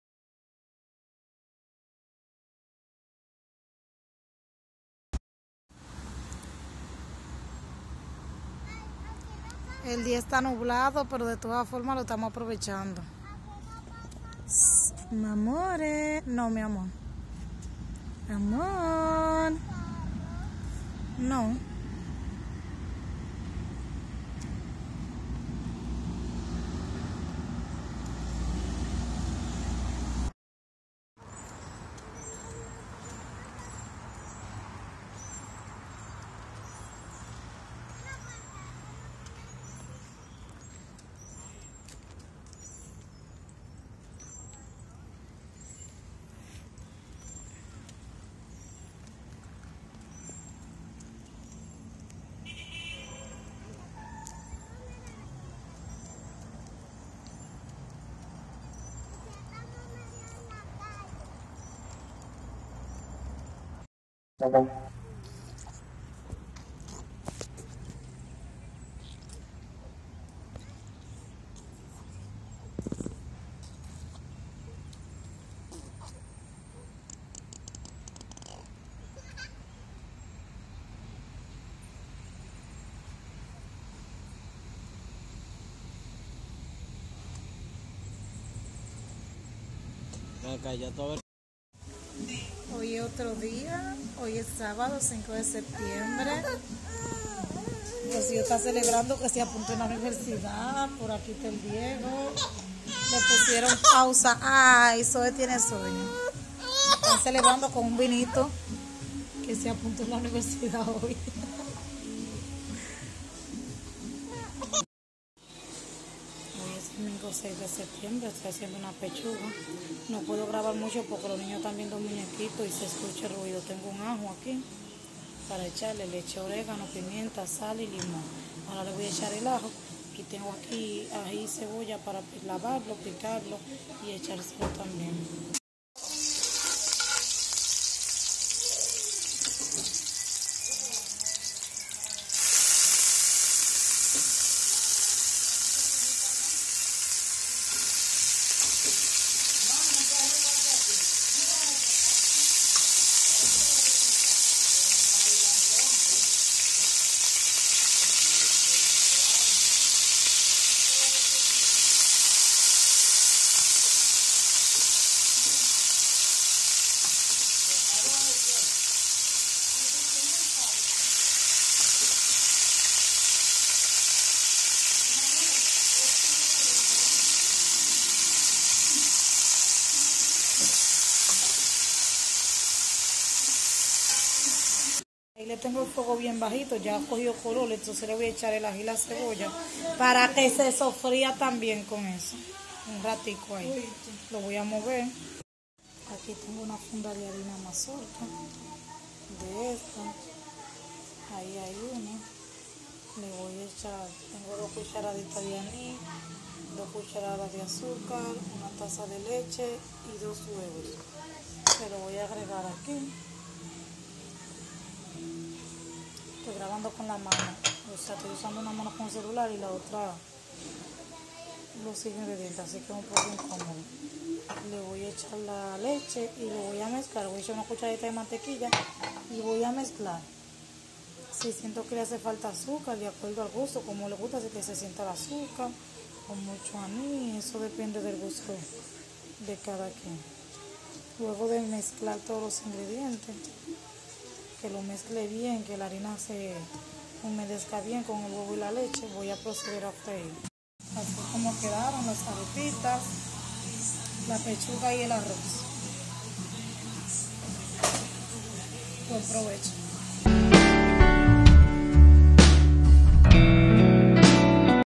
Vlogs/En el parque/Compartiendo/Cocinando
Les presento unos clics que grave con mi familia algunos se perdieron pero quise poner lo poco que quedó